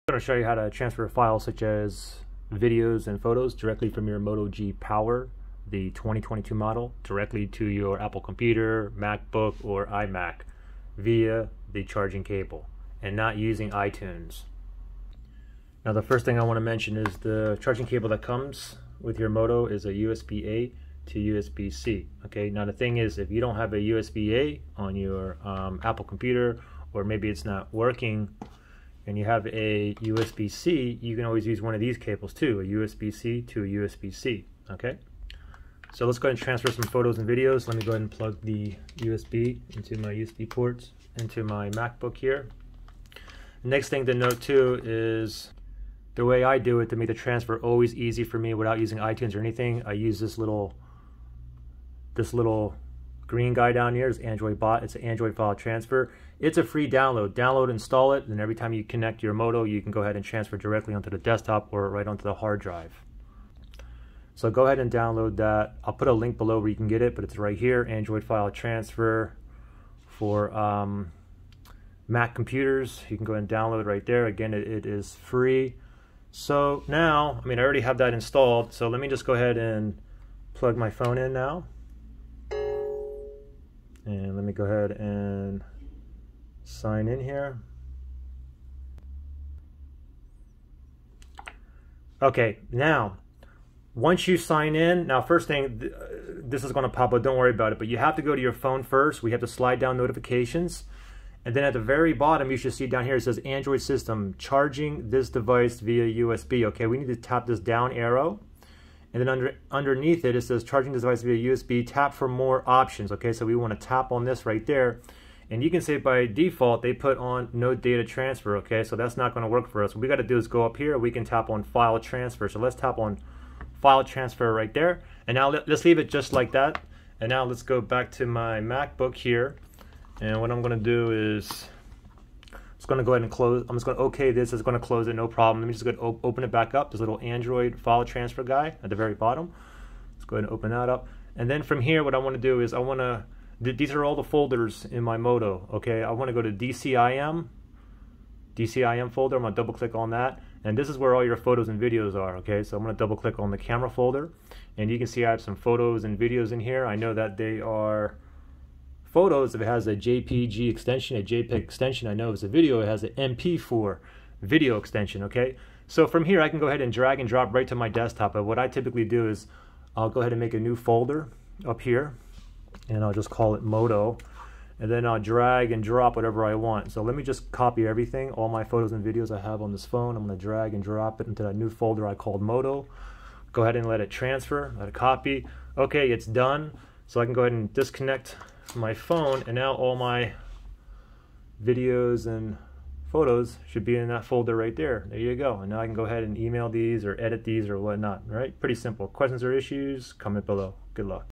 Moto G Power (2022): How to Copy Photos & Videos to MacBook, iMac, Apple PC w/ Cable (No iTunes)
I show you how to transfer (copy, move, download) files like photos and videos directly from a Moto G Power (2022) smartphone to a MacBook, iMac, Apple Computer via cable and NO iTunes. Hope this helps.
Moto G Power | 2022 | Unlocked | Motorola
USB-C to USB-C Cable [2-Pack 6.6ft]